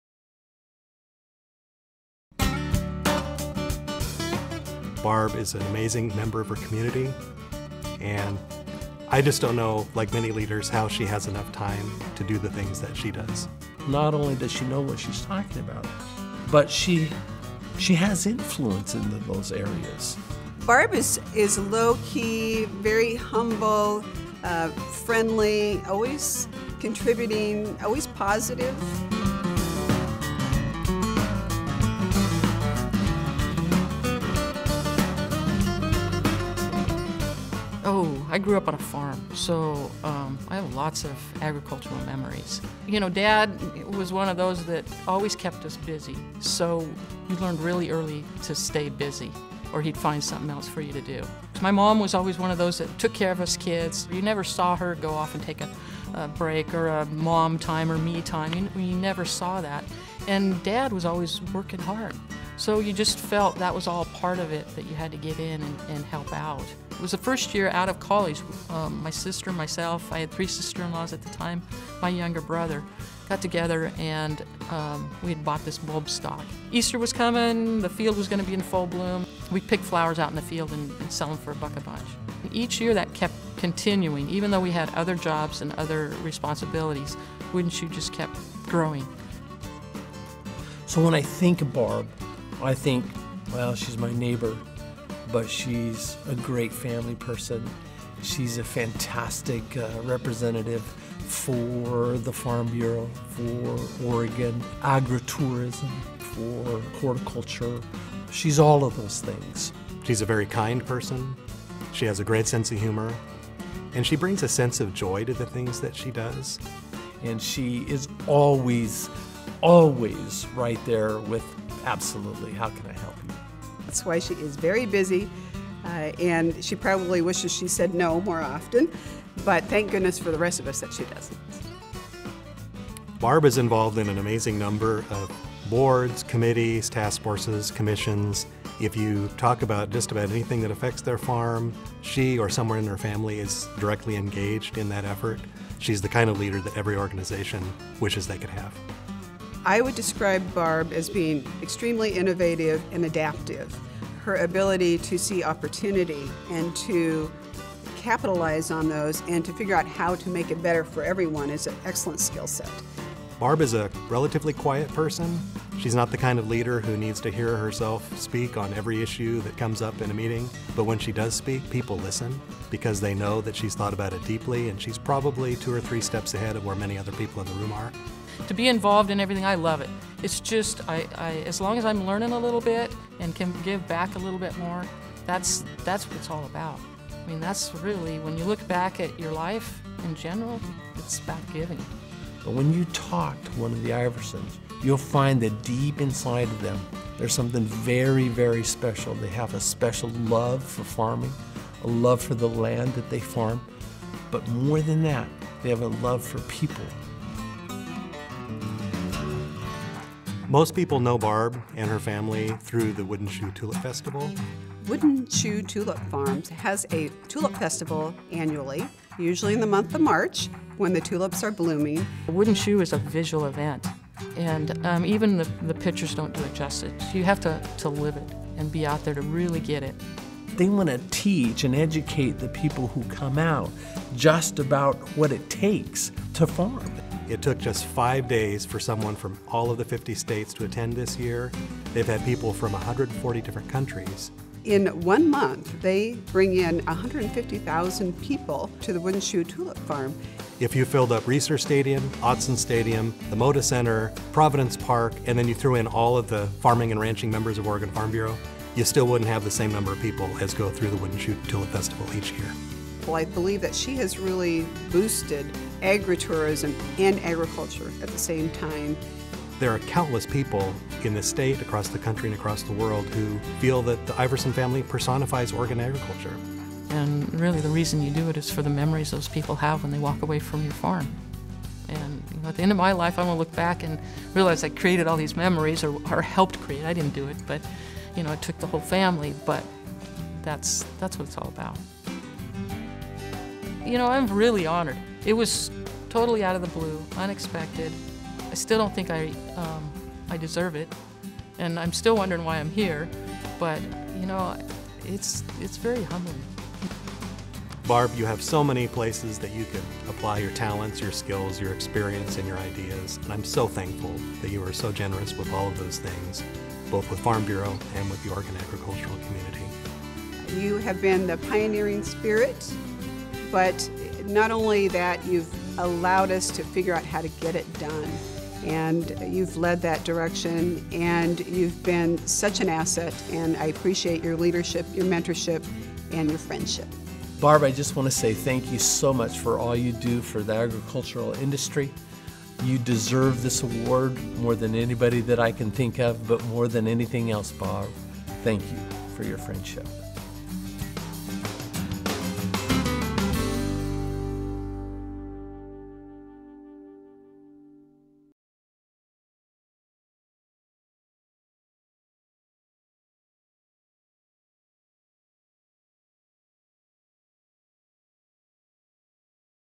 Barb Iverson - 2015 Agriculturist of the Year
Barb Iverson was awarded the Agriculturist of the Year award at Oregon Aglink's 2015 Denim & Diamonds Dinner and Auction.